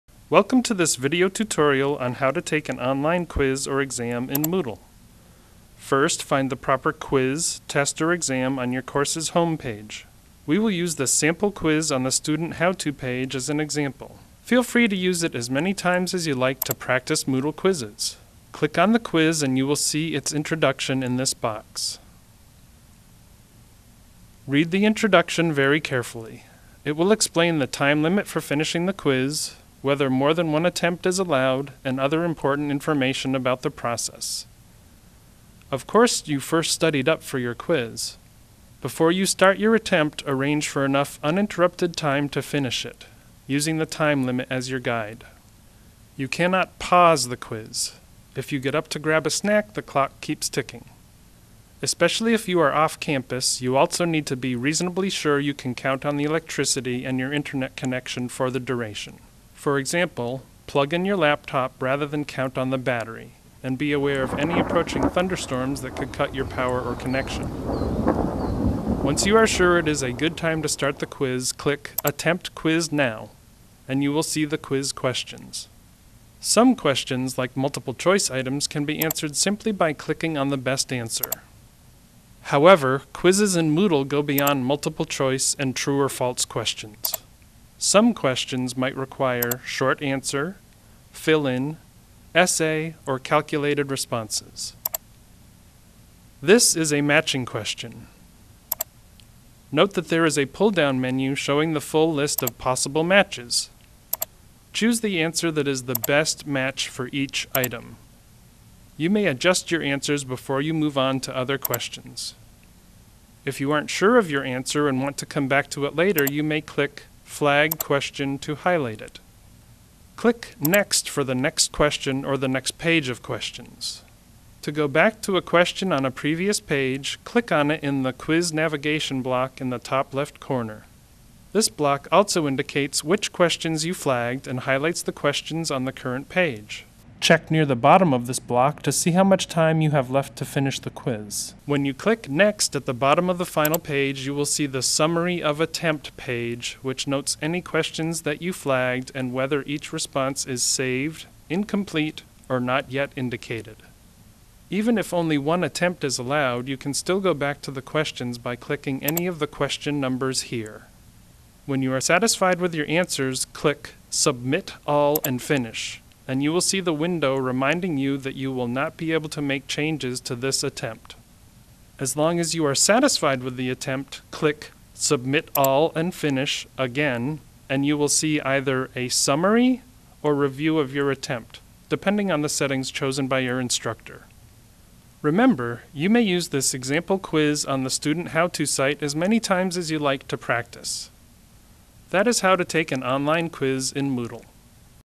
How to Take an Online Quiz
This brief tutorial explains how to take an online quiz or exam in Moodle. Closed Captions are available by clicking on the CC symbol at the bottom of the player window.